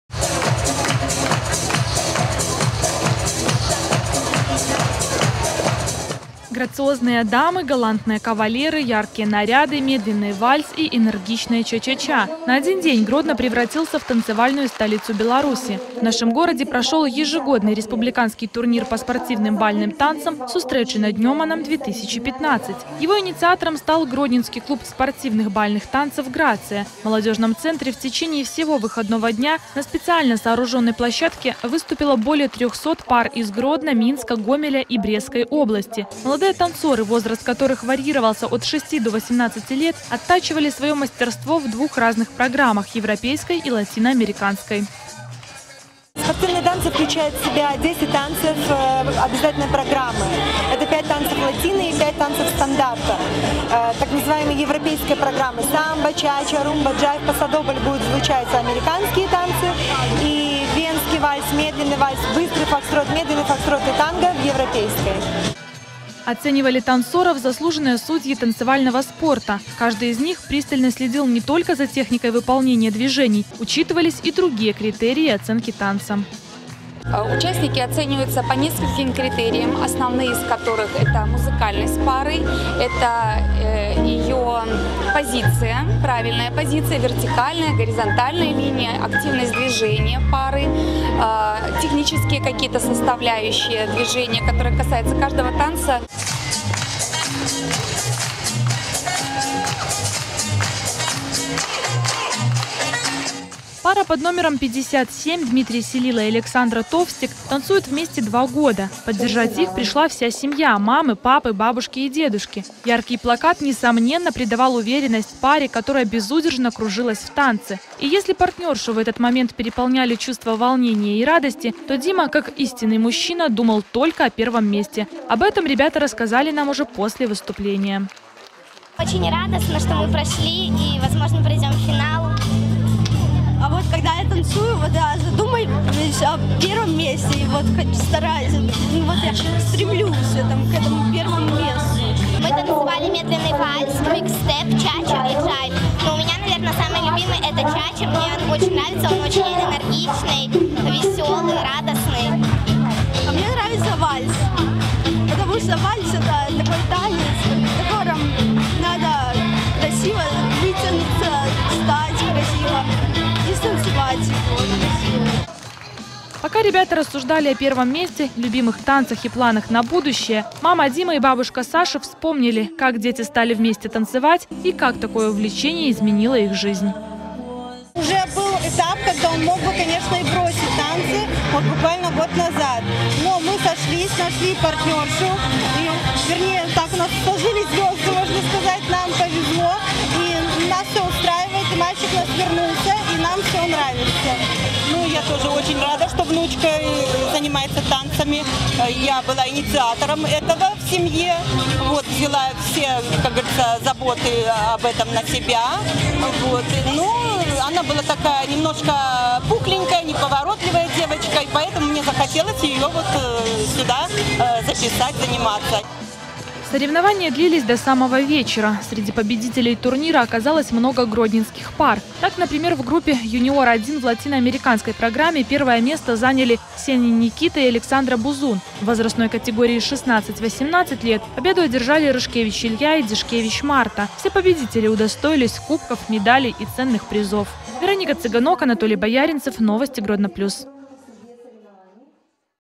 Сюжет Турнир по спортивным танцам
В Гродно прошел республиканский турнир по спортивным бальным танцам «Сустрэчы над Нёманам-2015», подробности на сайте